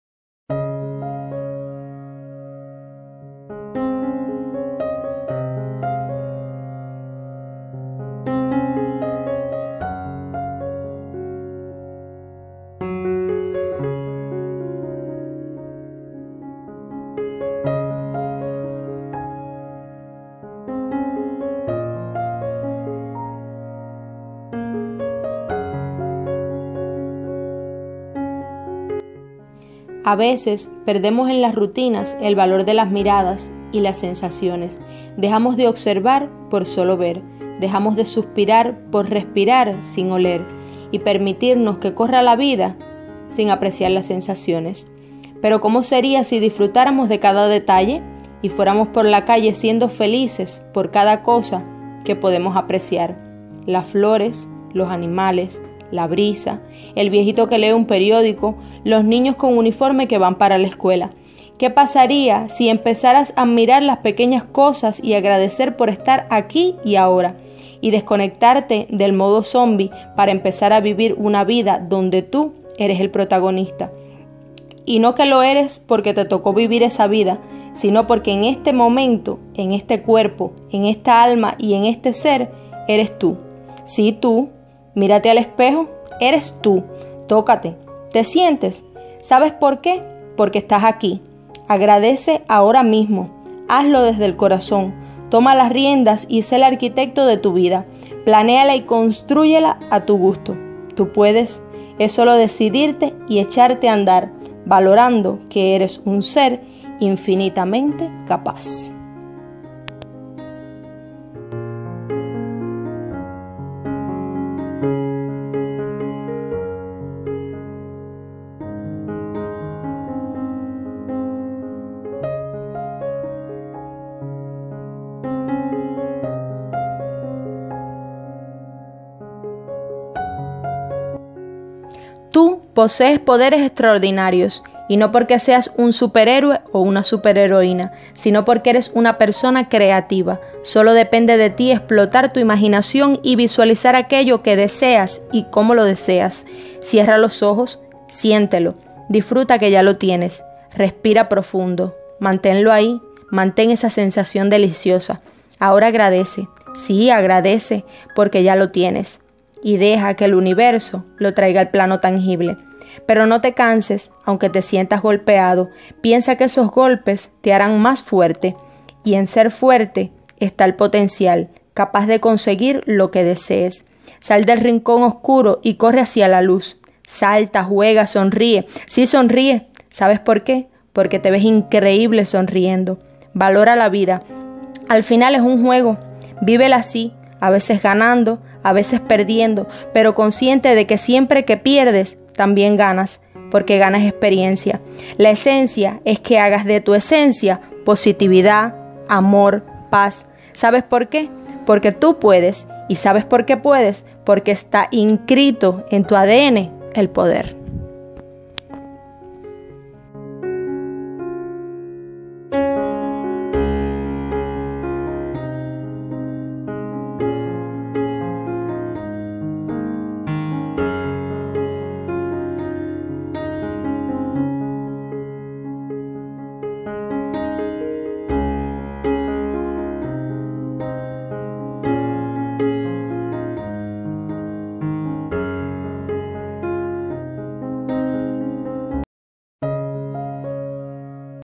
👉Haz esto para SALIR ADELANTE👈/Cómo ELIMINAR la ANSIEDAD 🤔Claryta/Pelo suelto y carretera
😎Mis amores! en esta ocasión les traigo un video inspiracional para salir adelante y eliminar la ansiedad,que les ayudará a potenciar su positivismo.Espero les encante un beso enorme desde el fondo de mi corazón💋